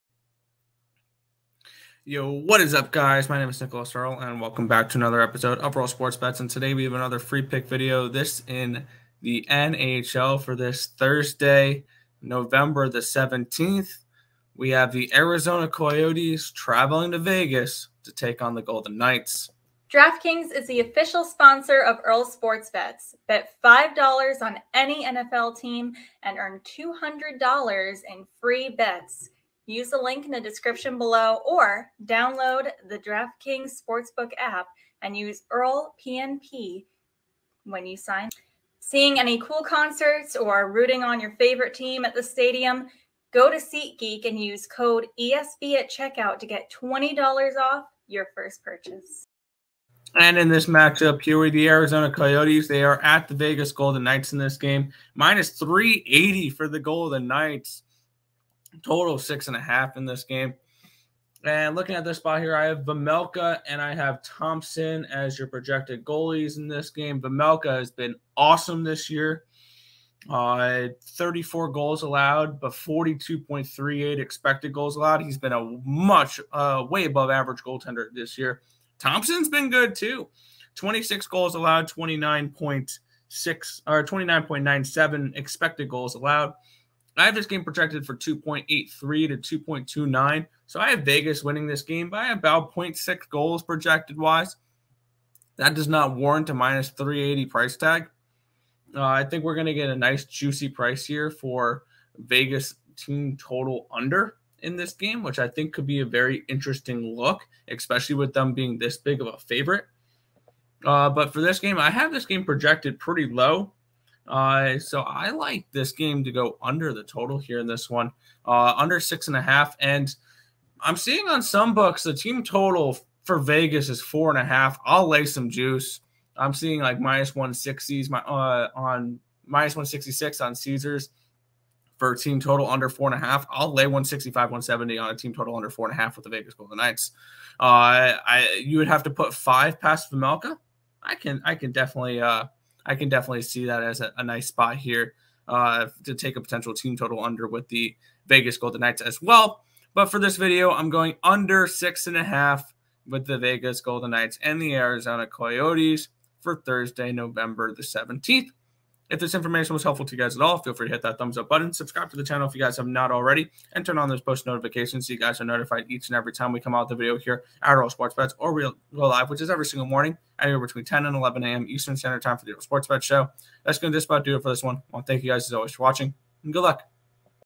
NHL Free Pick For November 17th, 2022- Arizona Coyotes @ Vegas Golden Knights | Earle Sports Bets
DK Promo- Bet $5 on ANY NFL Team Get $200

Earlesportsbets.com/promo

Picksandparlays.net